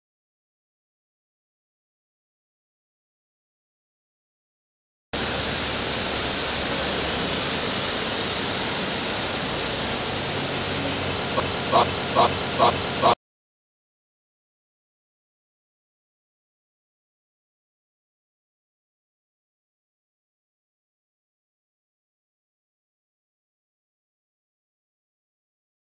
No TP on a Thai Shitter!
A long, squashed bus trip through Thailand after eating scorpion the night before can only lead to one thing...